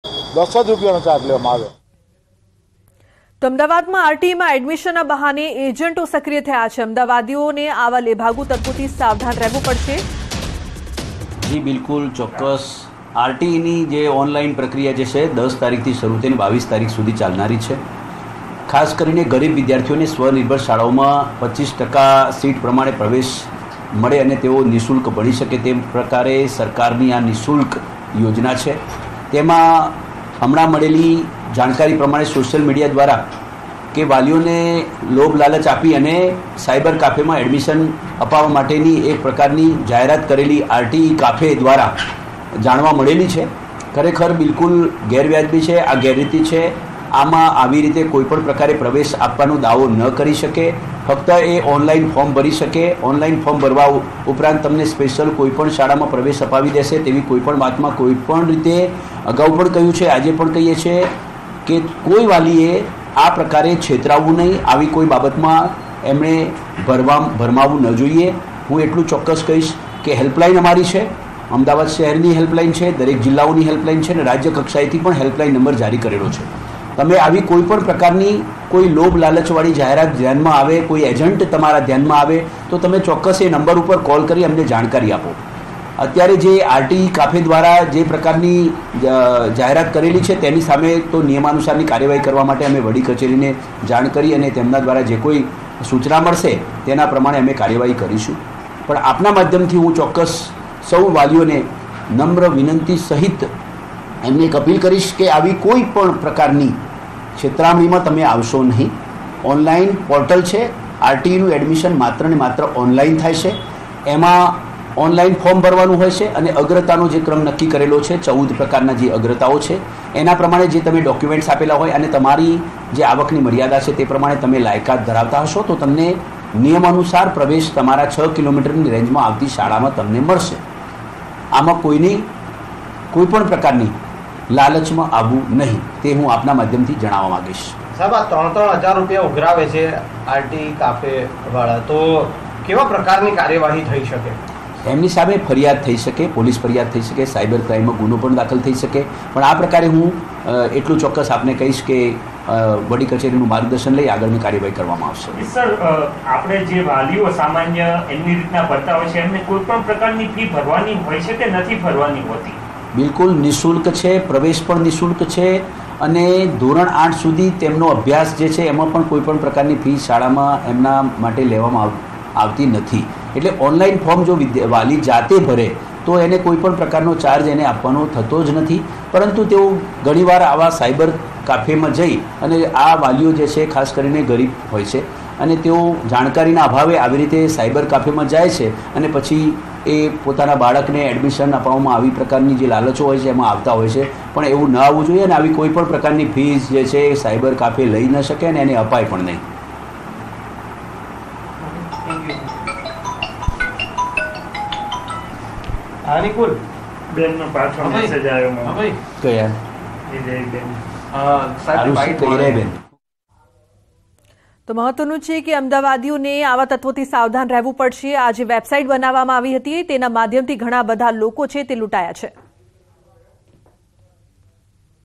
Ahmedabad News: RTE માં એડમિશનના નામે ઉઘાડી લૂંટ | Gujarati News | News18 Gujarati
Ahmedabad News: Robbery on the pretext of admission in RTE. Open robbery in the name of admission in RTE.

Ahmedabad News: RTE માં એડમિશનના બહાને લૂંટ. RTE માં એડમિશનના નામે ઉઘાડી લૂંટ. n18oc_ahmedabad